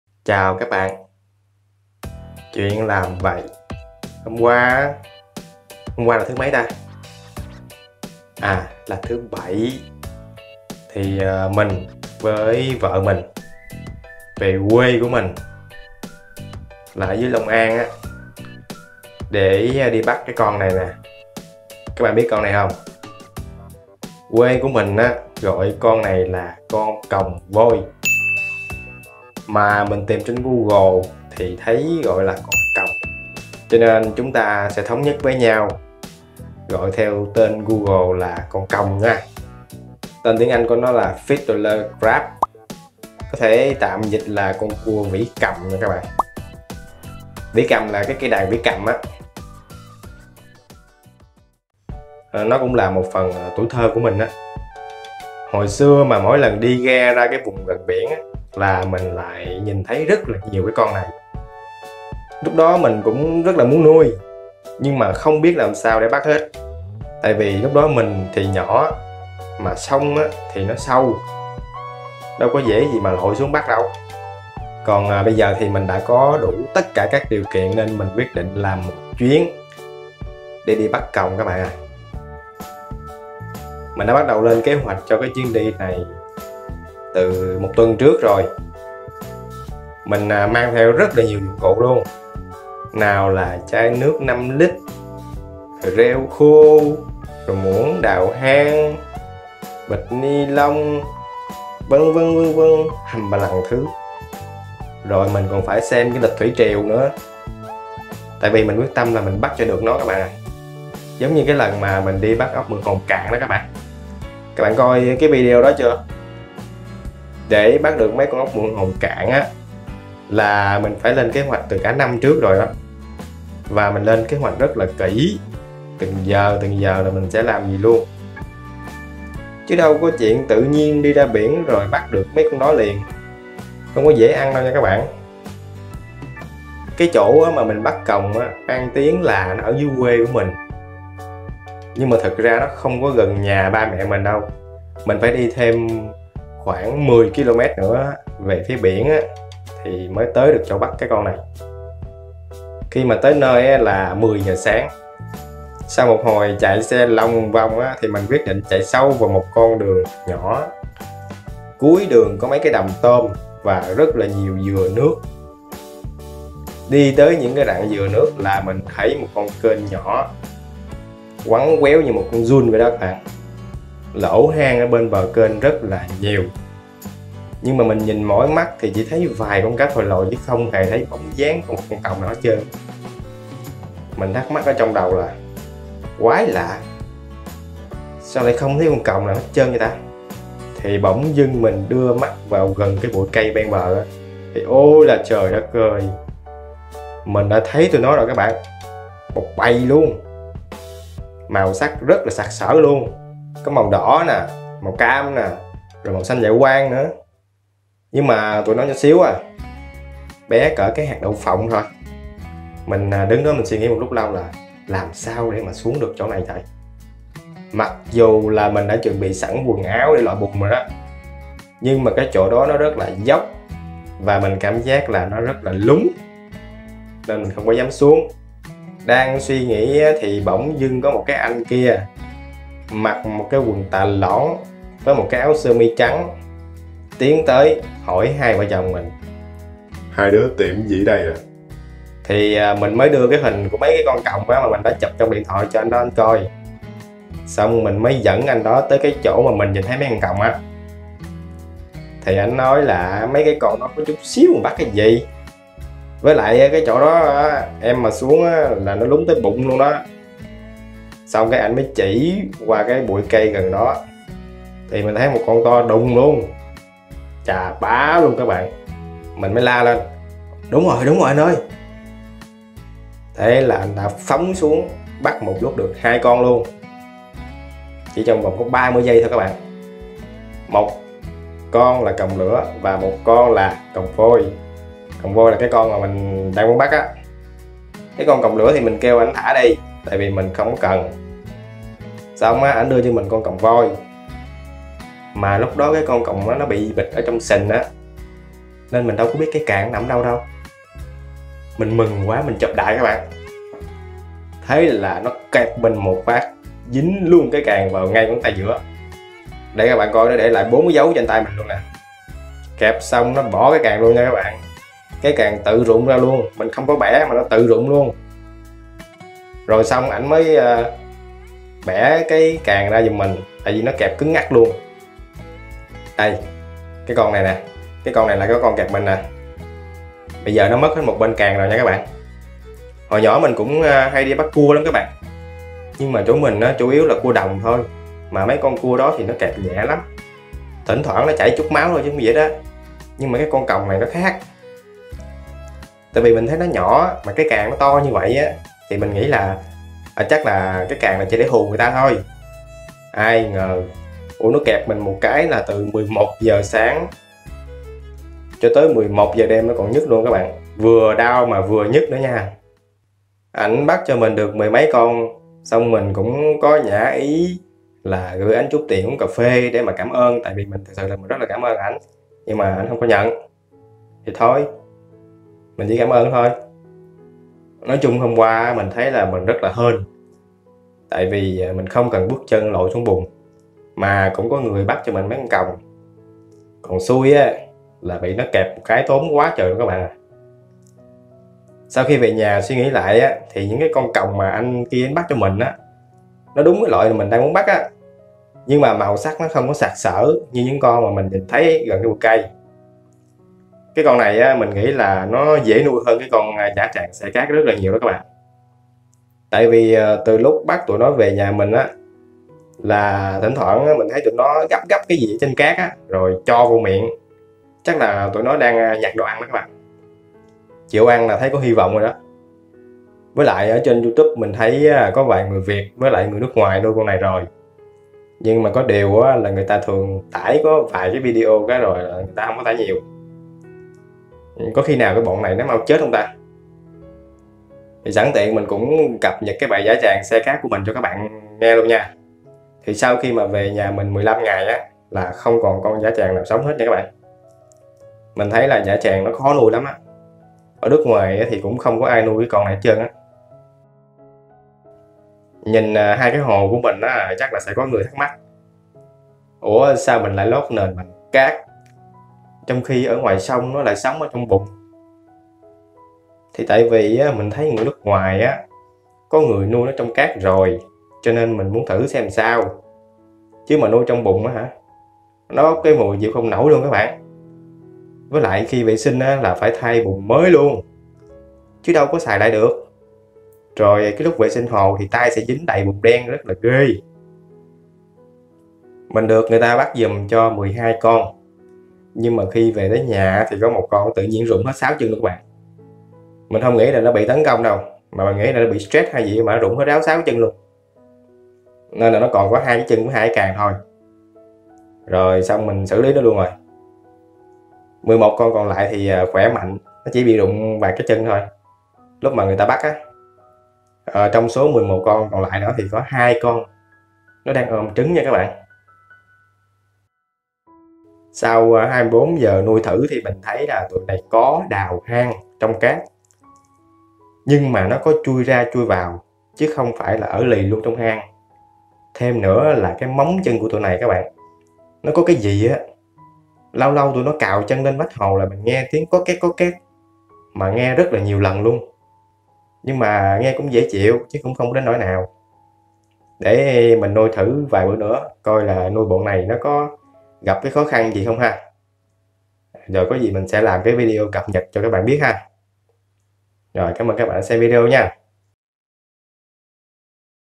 Lần đầu nuôi thử Con Còng và bị kẹp thủng 4 lỗ | Nuôi con Còng Vôi, Còng Bảy Màu | Fiddler Crab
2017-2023 Ba Con Rồng Channel.